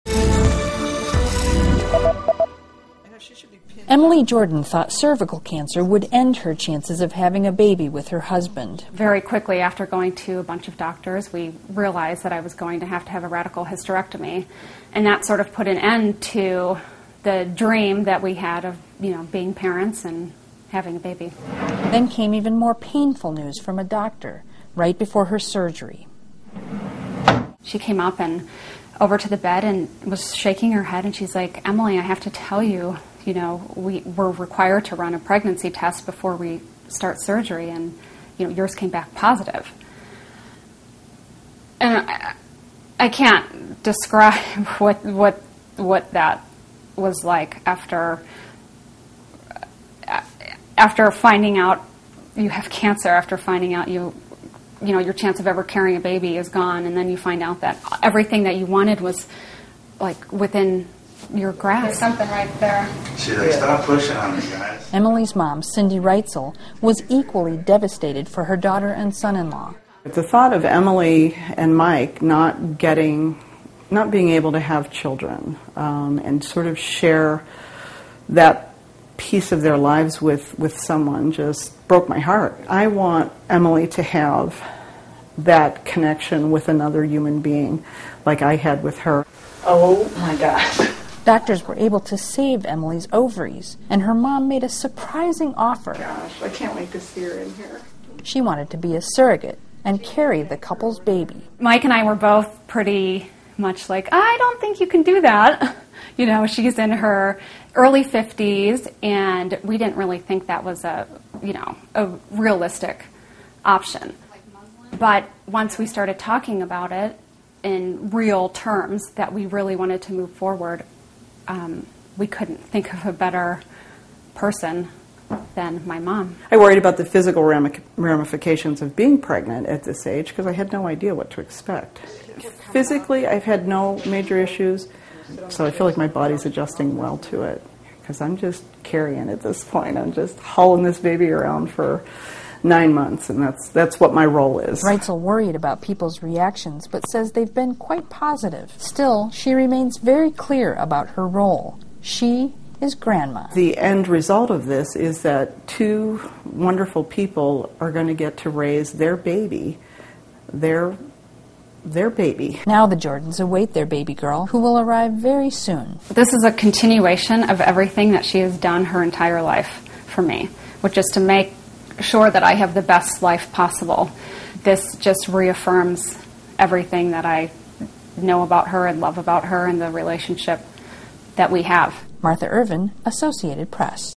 Grandma to Give Birth to Her Grandchild
Faced with cervical cancer at a young age, Emily Jordan underwent a radical hysterectomy in 2010. She thought she and her husband would never have their own children. Then her mother made a surprising offer. (Aug. 28)